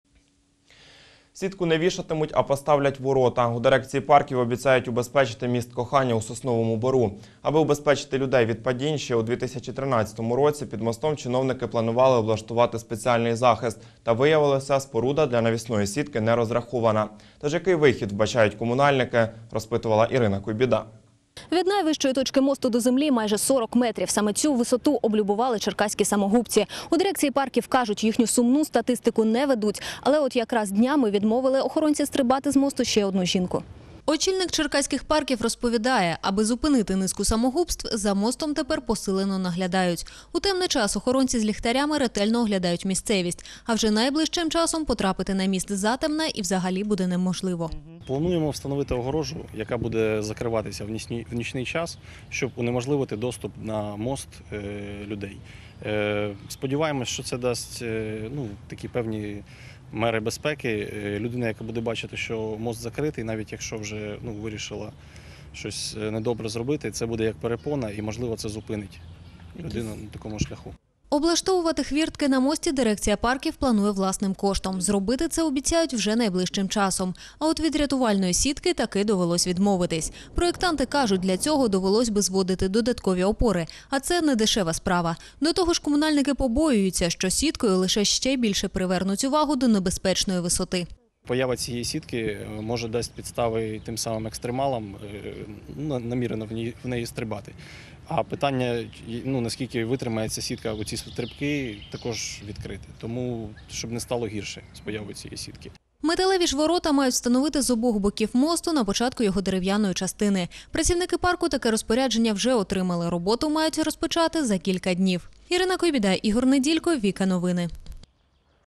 Владці обіцяють убезпечити черкаський міст Кохання
Сітку не вішатимуть, а поставлять ворота